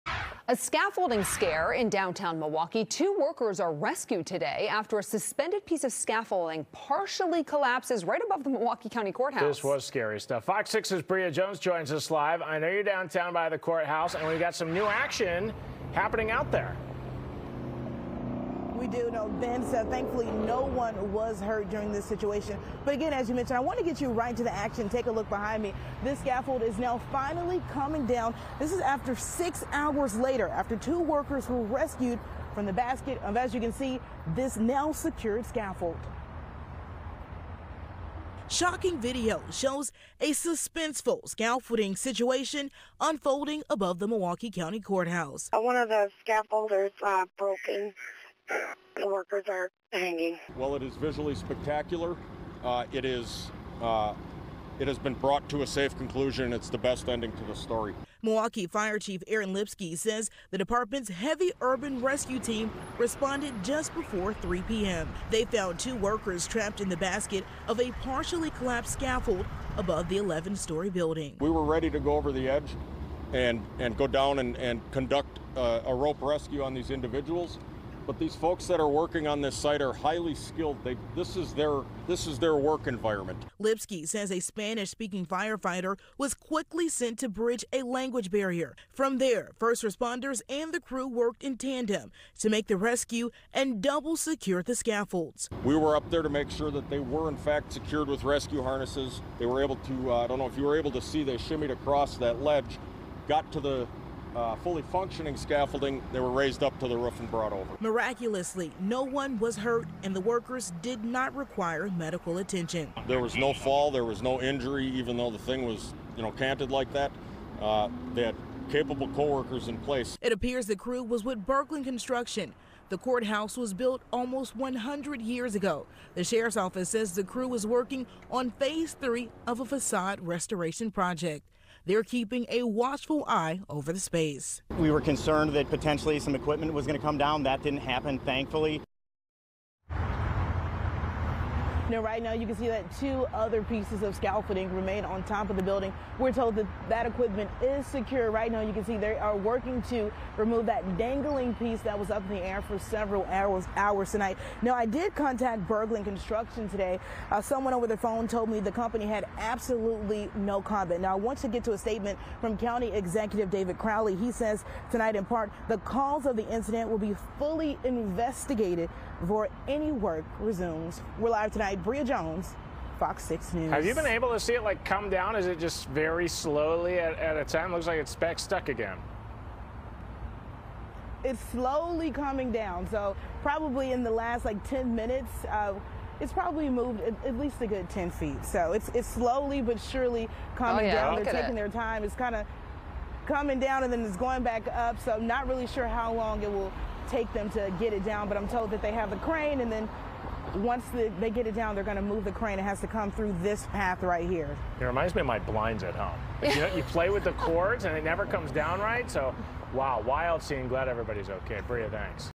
Workers rescued from Milwaukee County Courthouse scaffolding | FOX6 News Milwaukee
Two workers were rescued after the scaffolding they were on partially collapsed and ended up in a precarious position in downtown Milwaukee on Thursday, Oct. 10.